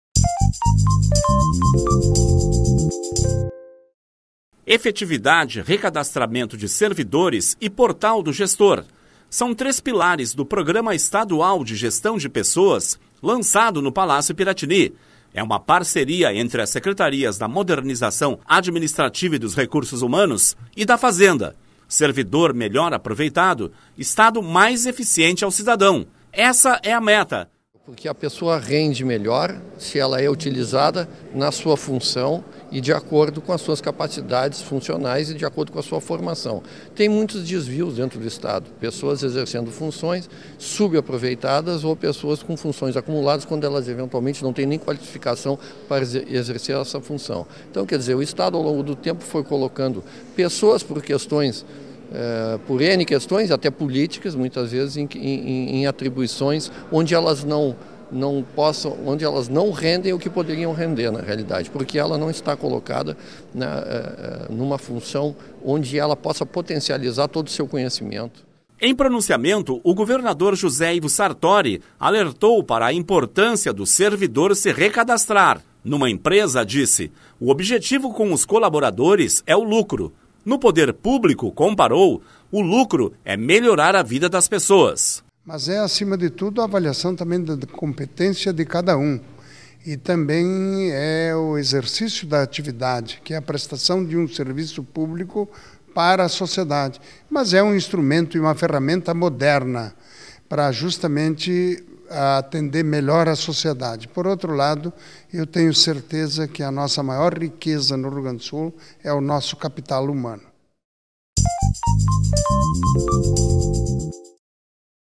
Programa de gestão qualifica gerenciamento de servidores do Estado
Efetividade, recadastramento de servidores e portal do gestor. São três pilares do Programa Estadual de Gestão de Pessoas, lançado no Palácio Piratini.